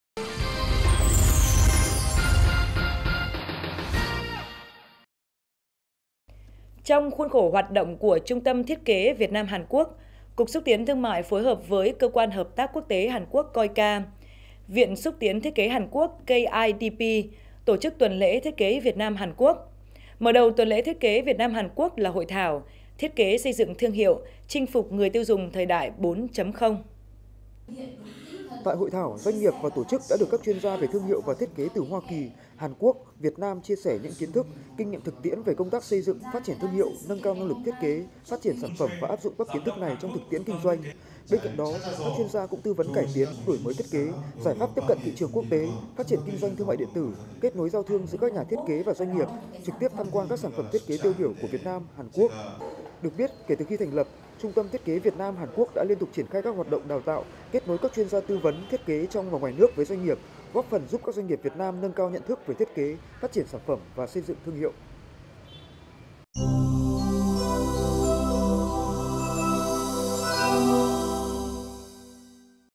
Thiết kế, xây dựng thương hiệu: Chinh phục người tiêu dùng thời đại 4.0
Trong khuôn khổ hoạt động của Trung tâm Thiết kế Việt Nam - Hàn Quốc, Cục Xúc tiến thương mại phối hợp với Cơ quan Hợp tác Quốc tế Hàn Quốc (KOICA), Viện Xúc tiến thiết kế Hàn Quốc (KIDP) tổ chức Tuần lễ thiết kế Việt Nam – Hàn Quốc. Mở đầu tuần lễ thiết kế Việt Nam – Hàn Quốc là Hội thảo “Thiết kế, xây dựng thương hiệu: Chinh phục người tiêu dùng thời đại 4.0”...